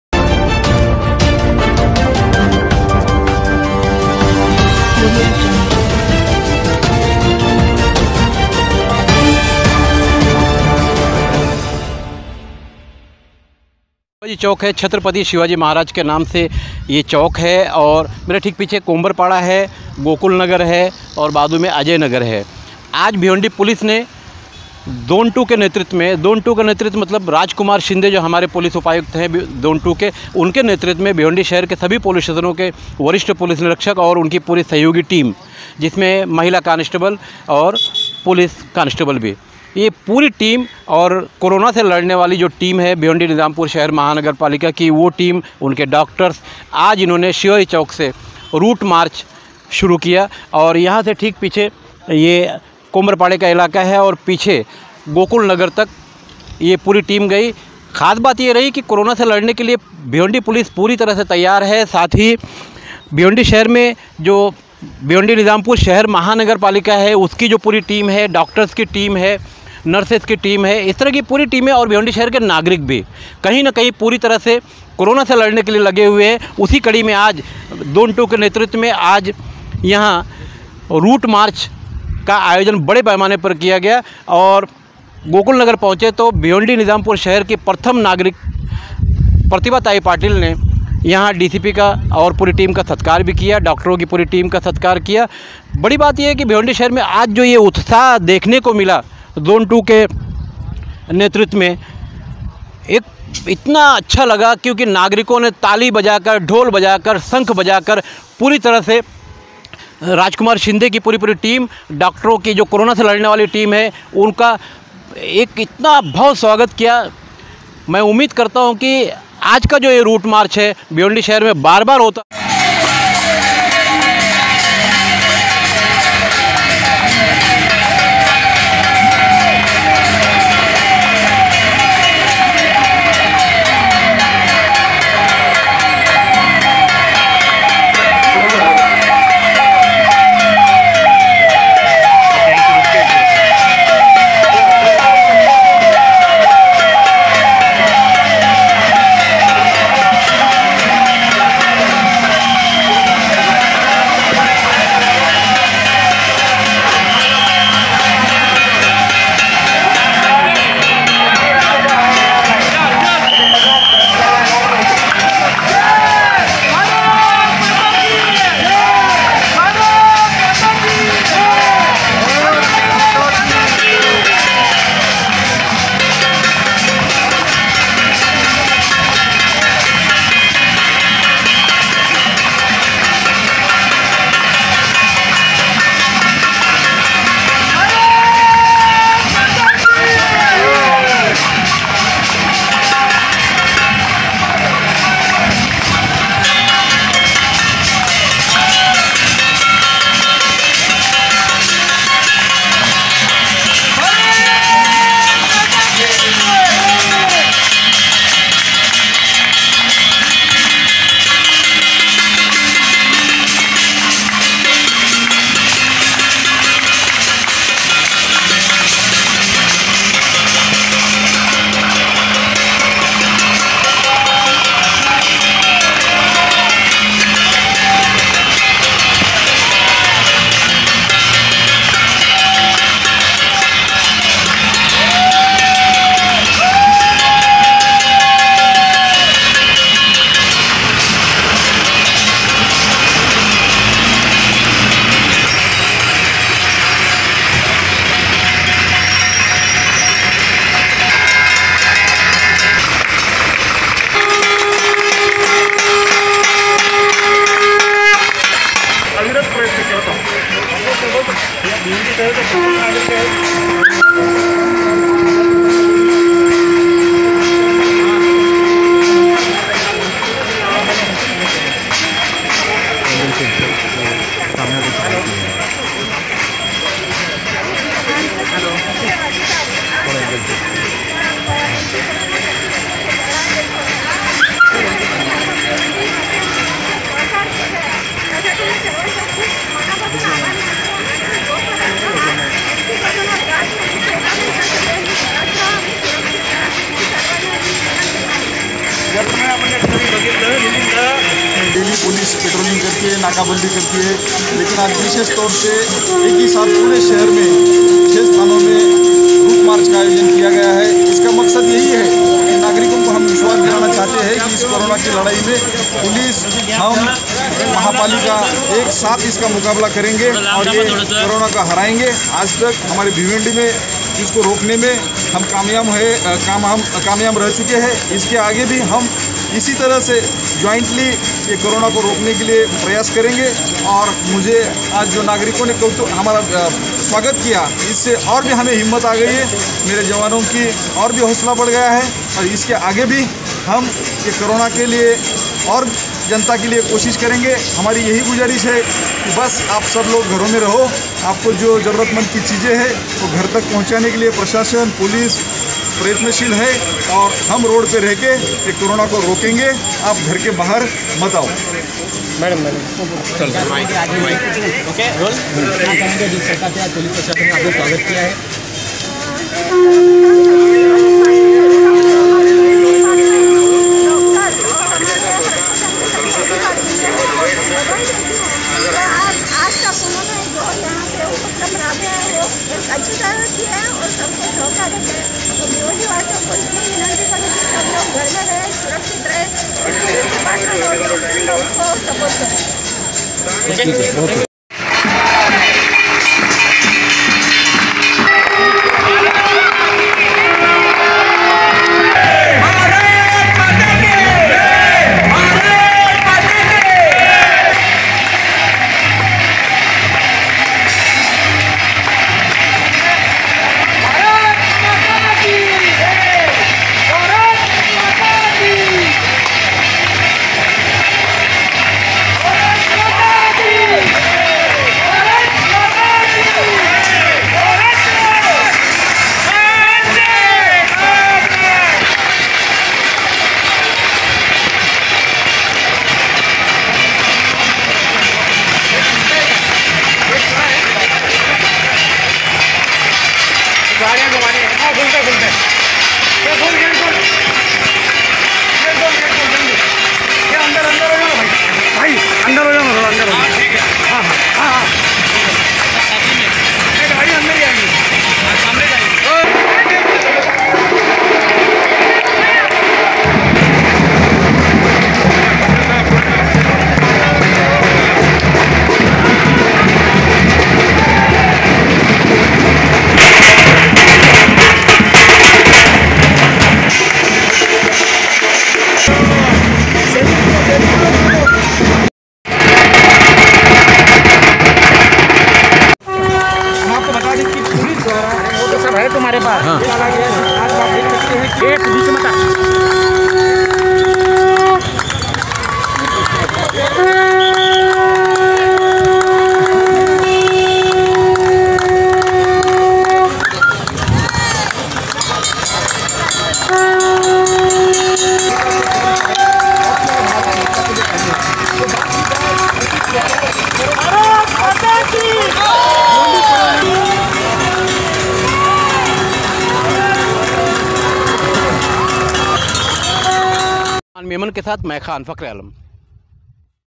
भिवंडी पोलिस ने किया धमाकेदार रुट मार्च,नागरिकों ने किया स्वागत,
भिवंडी पोलिस उपायुक्त राज कुमार शिंदे के नेतृत्व में भिवंडी पोलिस ने रुट मार्च किया, पोलिस और महानगर पालिका की पूरी टीम ने बढ़ चड़ कर भाग लिया,यह रुट मार्च शिवाजी चौक से कोंबड़ पाड़ा ,गोकुल नगर जैसे इलाकों में नागरिकों के भारी प्रतिसाद के साथ समाप्त हुआ,वीडियो,खान फखरे आलम,